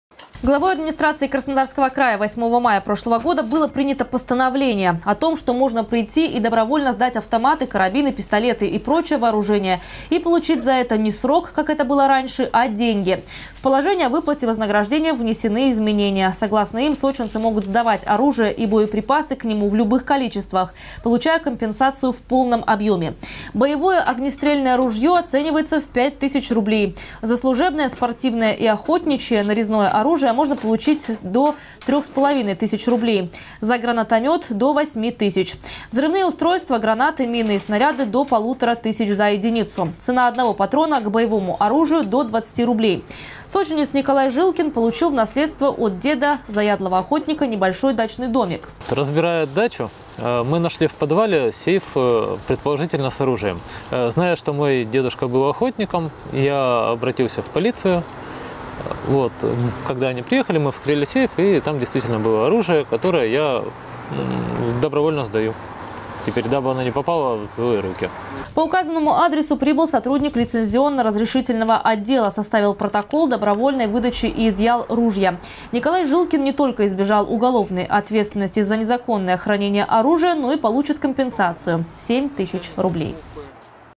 Сочинцам предлагают добровольно сдать оружие. Новости Сочи Эфкате
Главой Администрации Краснодарского края 8 мая прошлого года было принято постановление от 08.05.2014 о том, что можно прийти и добровольно сдать автоматы, карабины, пистолеты и прочее вооружение, и получить за это не срок, как было бы раньше, а деньги.
Сочи. Новости.
Телеканал Эфкате.